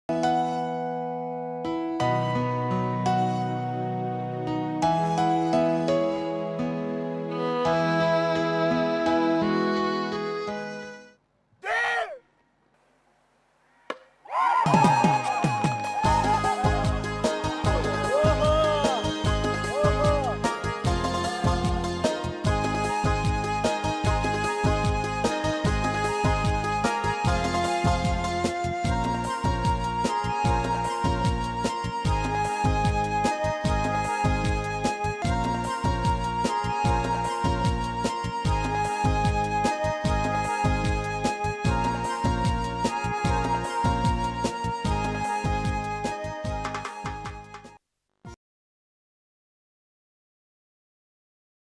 Yangphel Archery
Archery in Bhutan; Video of the most popular archery tournament in the Kingdom of Bhutan